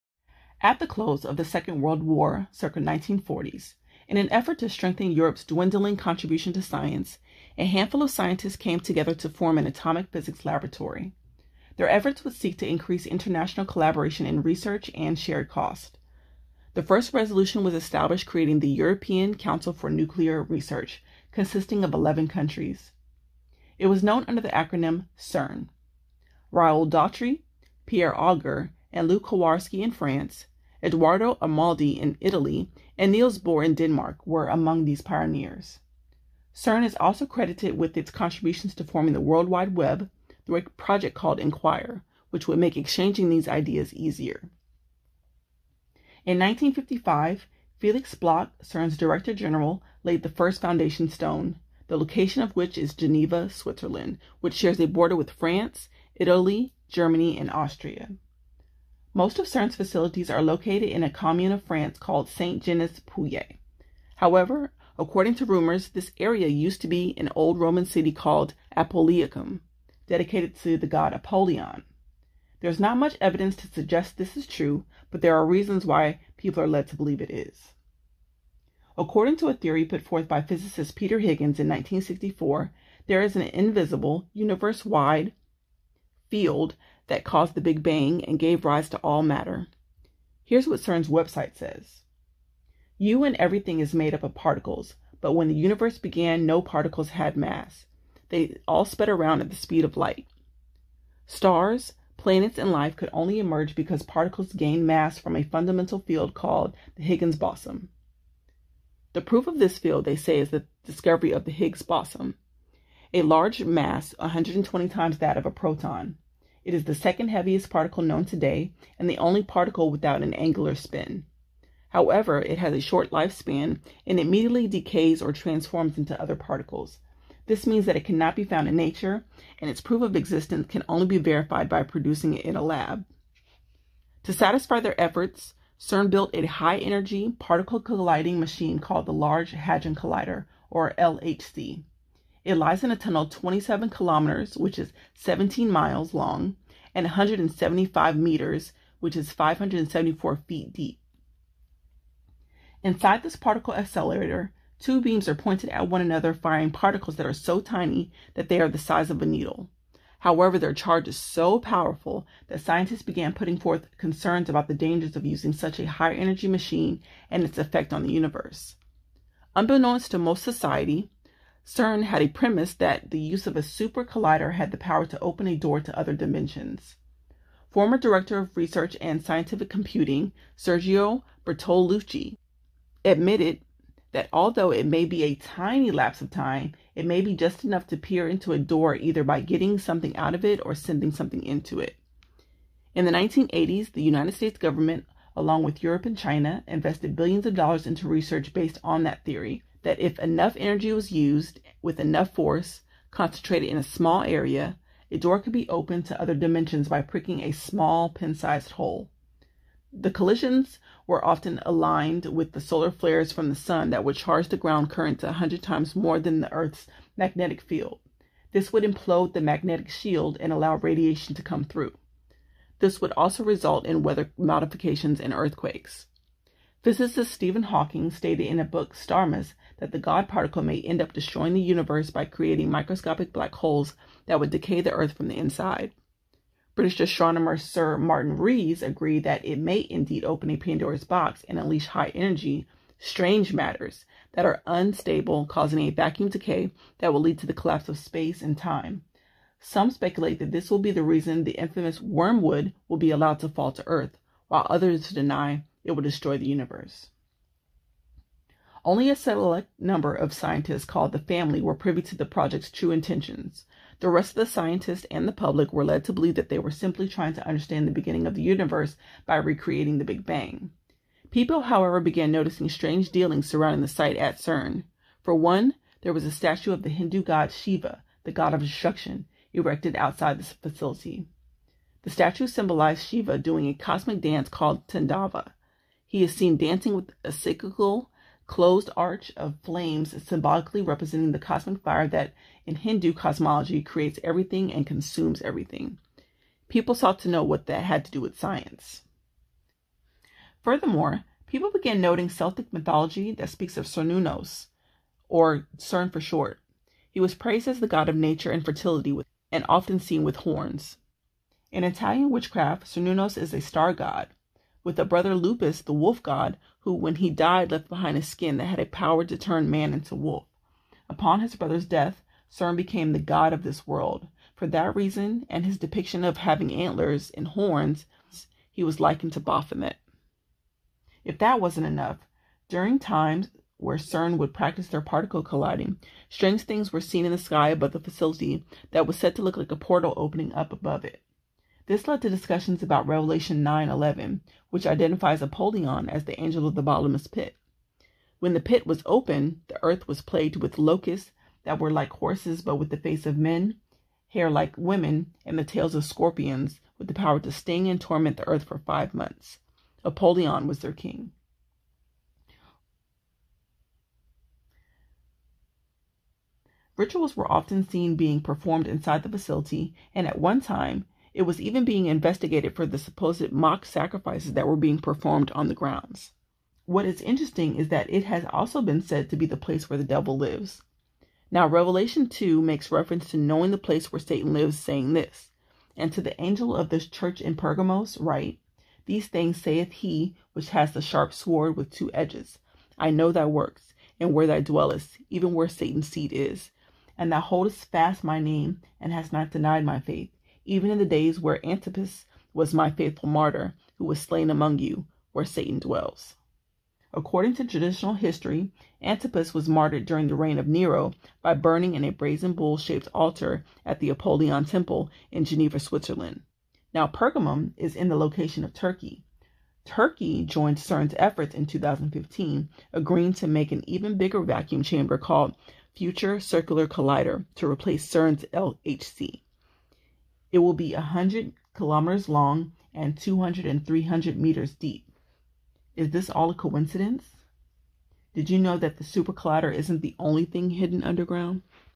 CERN, Other Dimensions, and Satan's Seat
Exploring the history of CERN, it's true intentions, and why it's said to be the place where Satan dwells.

To support: 💰
$Taydollafasho